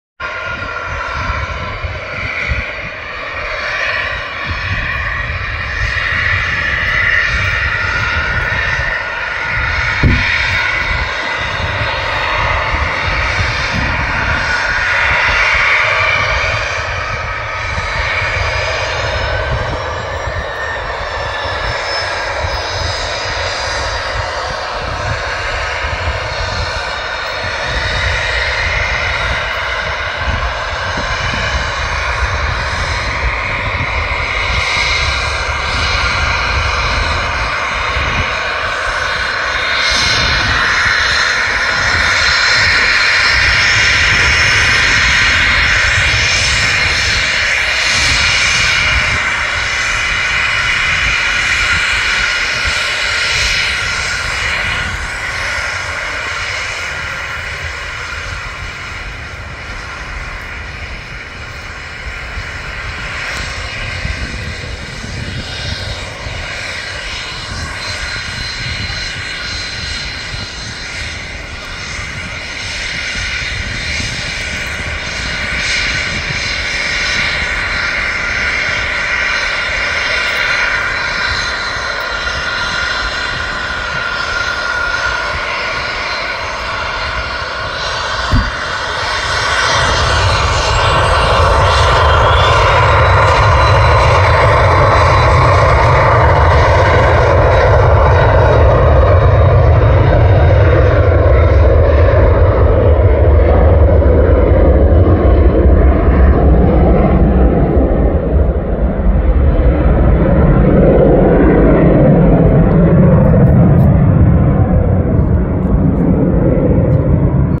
Beautiful Embraer Phenom 300E take-off. Shoot by Samsung S23ultra
Check out this captivating video of the stunning Embraer Phenom 300E taking off, beautifully captured by the Samsung S23 Ultra. Experience the sheer power and elegance of this aircraft as it dominates the skies, with the added thrill of the thunderous roar of its engines.